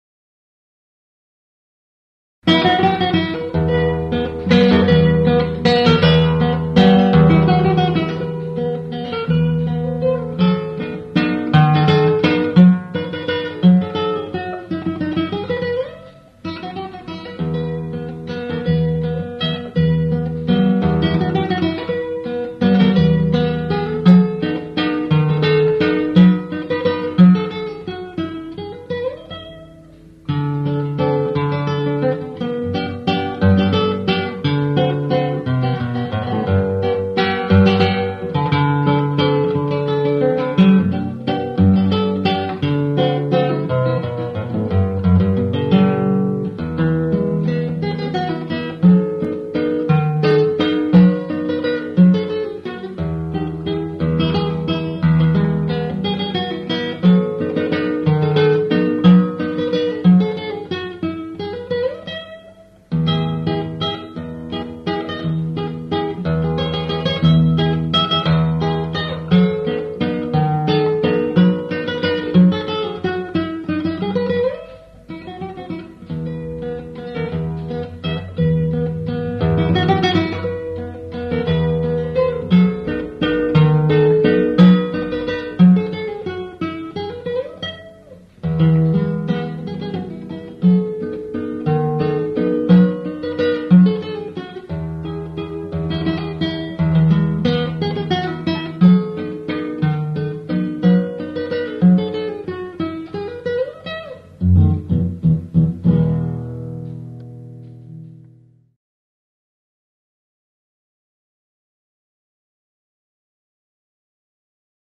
Horacio Aguirre en vivo - Tema instrumental
Grabación en vivo de 1988 que me paso Claudio Salinas
Un gran amigo que difundio la Obra de Los Cantores del Alba, partio al cielo en Abril de 2010.